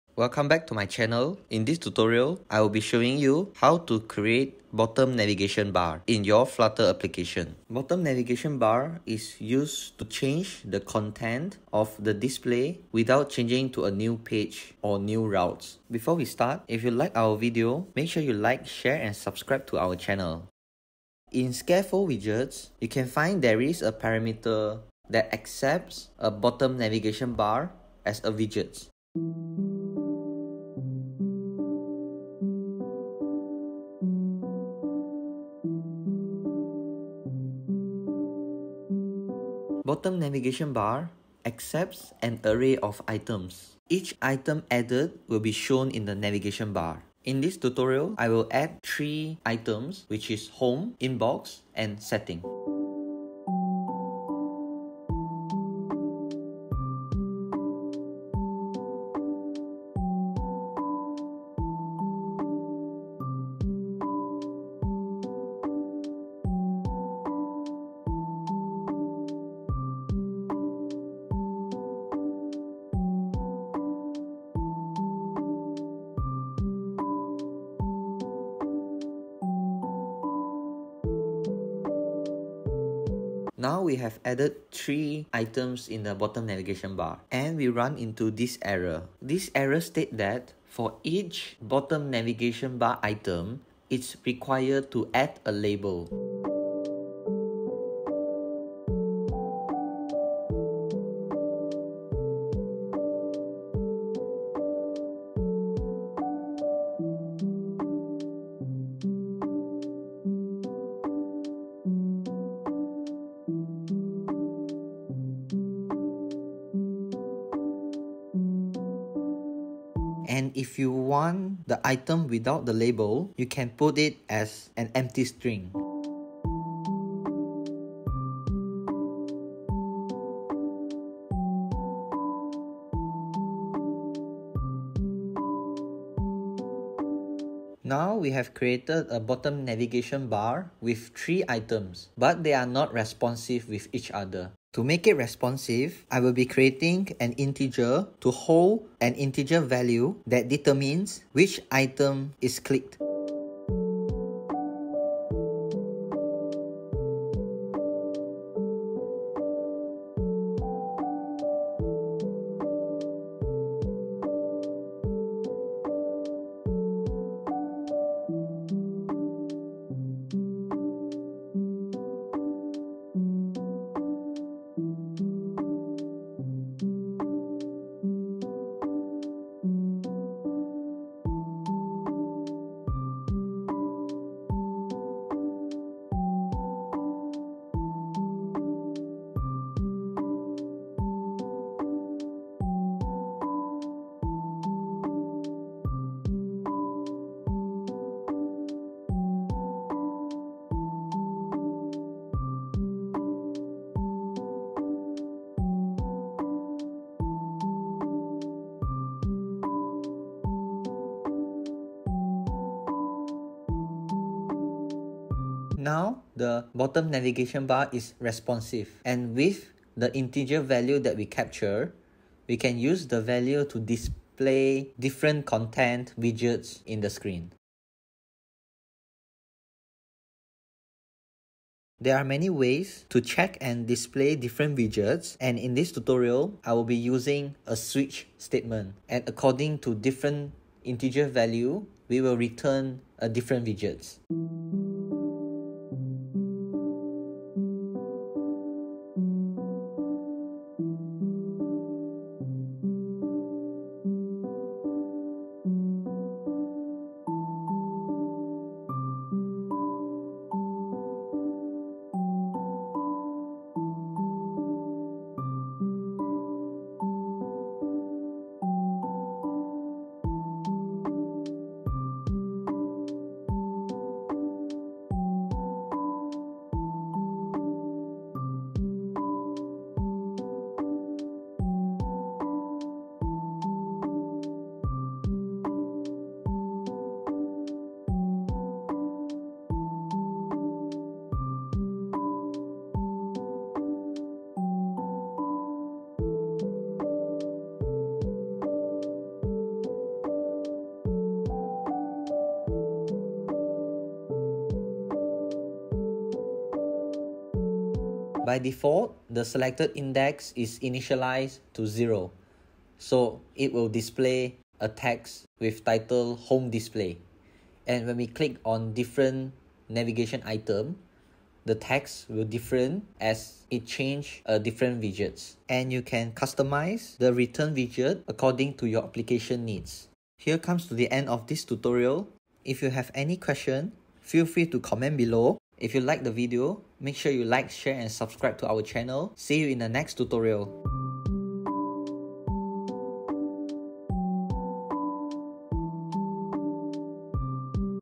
How to create bottom navigation bar in flutter in 5 minutes
This tutorial shows you how to create bottom navigation bar in your flutter application. In this flutter tutorial, we will display steps to create a bottom navigation bar from scratch.

The main use of bottom navigation bar is to allow user to scroll through different navigation bar items without changing routes or pages.